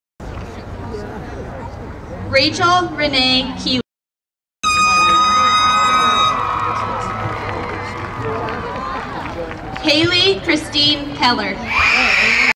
Rachael Renee Keeler
Fruita Monument High School Graduation in Grand Junction, Colorado on May 12, 2015.